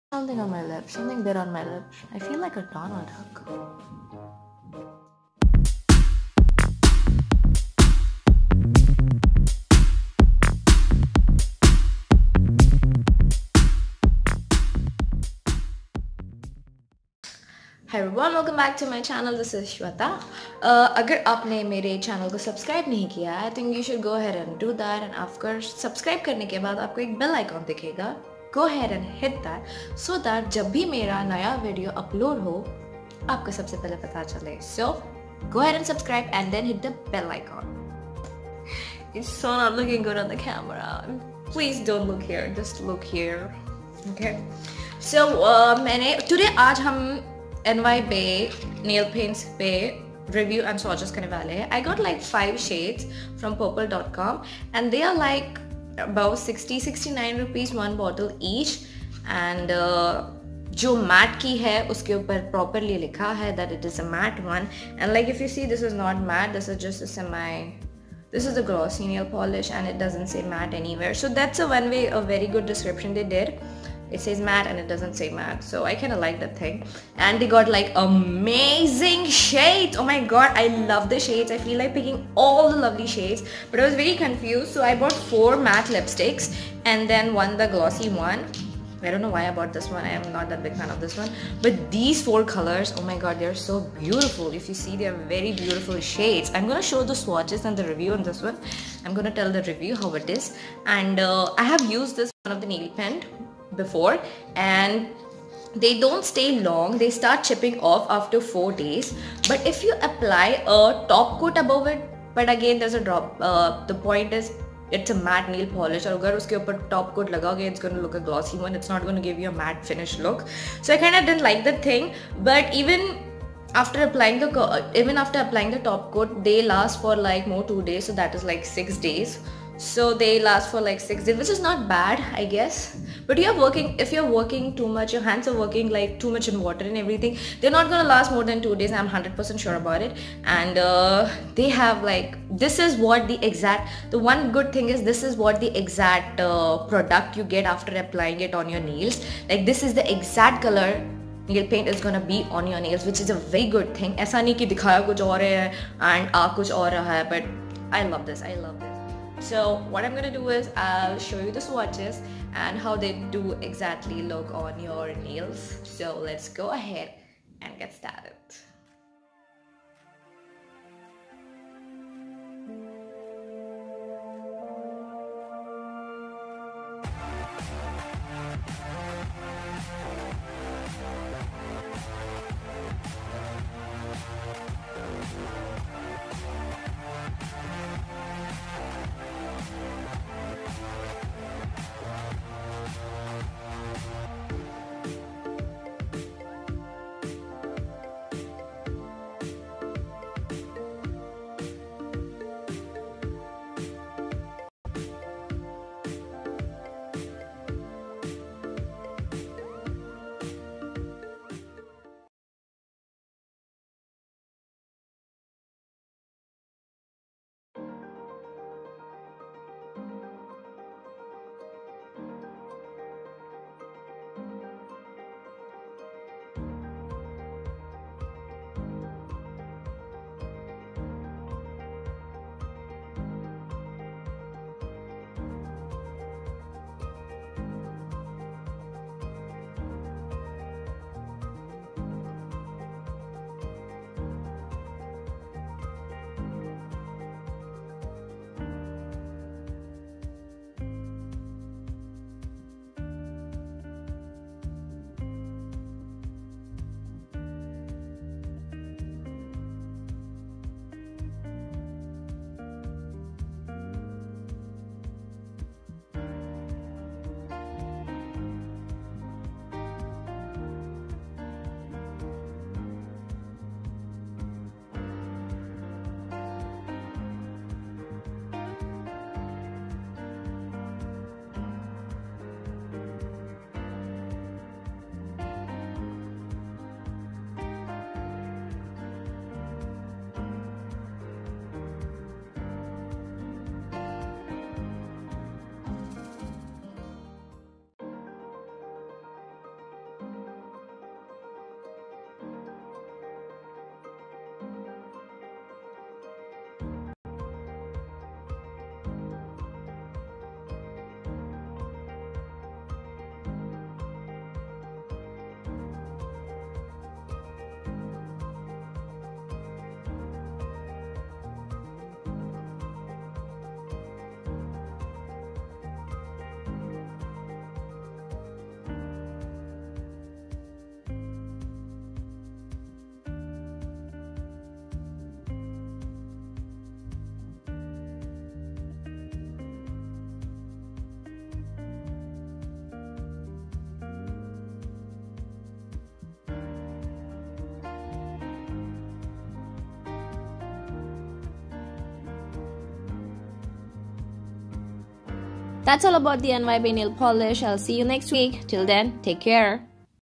Affordable and good quality NYbae nailpolish swatches/review
What should I say about these...
Color is exactly how the bottle looks like
Perfect matte nailpaint
Very affordable like 69rs ONLY
Plus they don't make your nail yellow..
Not toxic..
They have variety of lovely shades
But they dont last more than days..they start chipping off on 4th day..